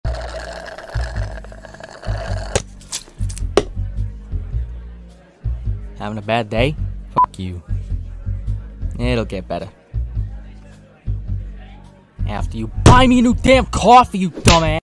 bad day (ORIGINAL SHORT ANIMATION)
it's been one of those days

starring Blitz and Game
animation, music, and voices by me